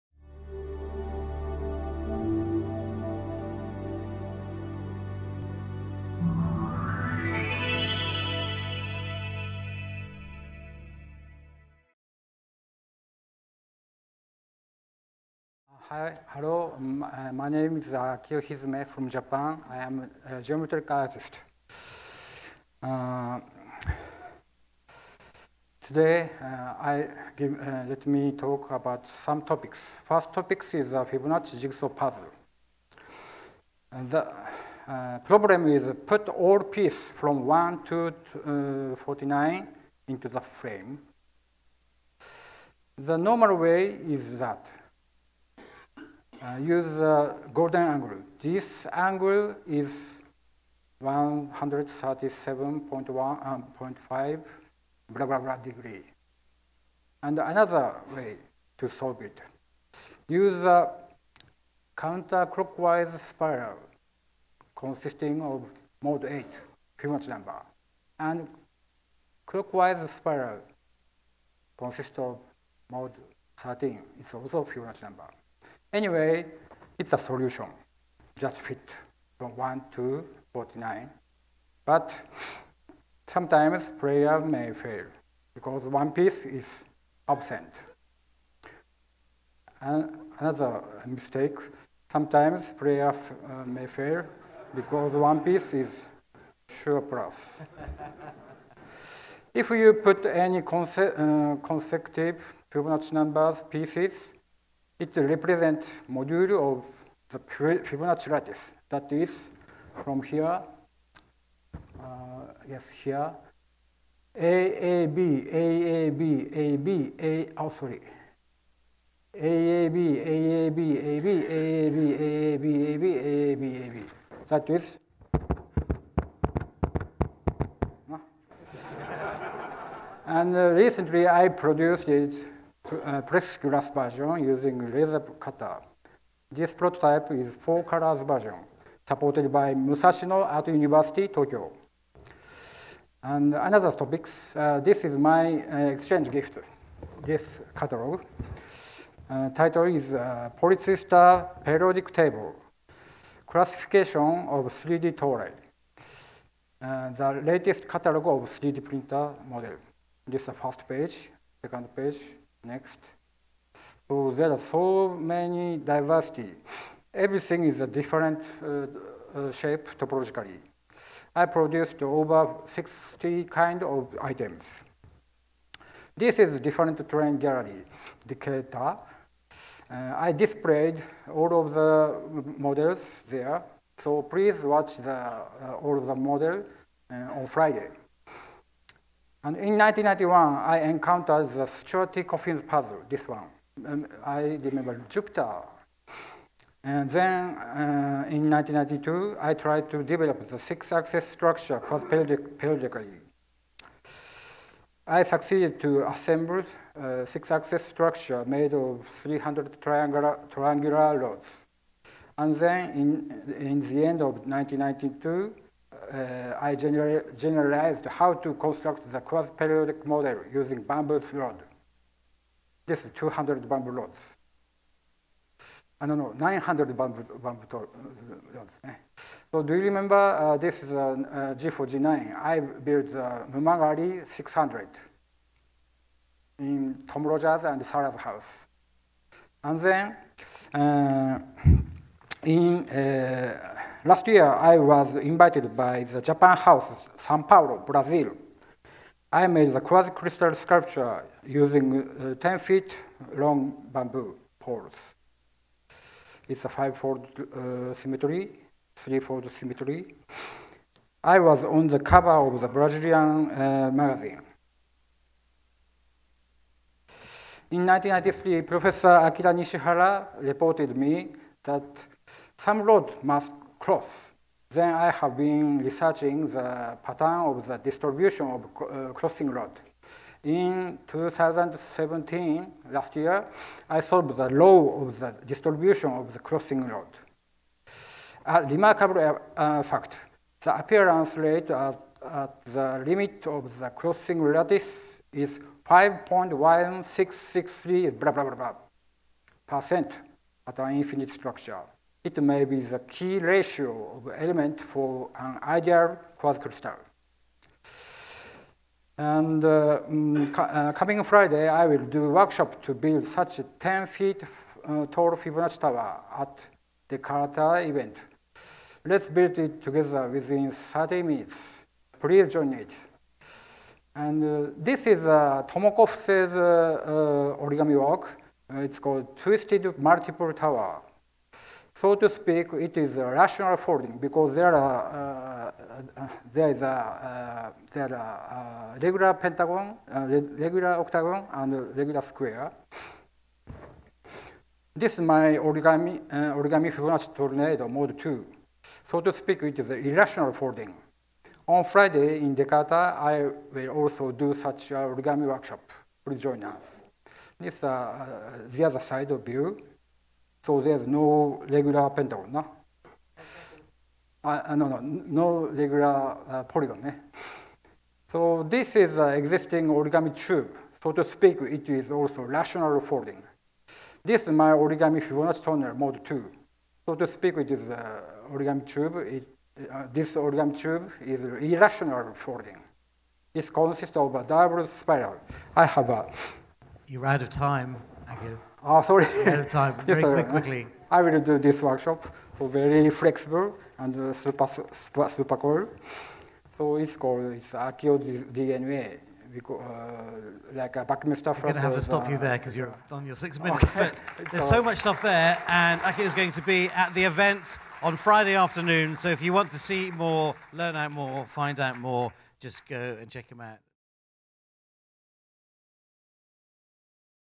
Akio Hizume - Fibonacci Jigsaw Puzzle etc. - G4G13 April 2018
I show new attempts after G4G12.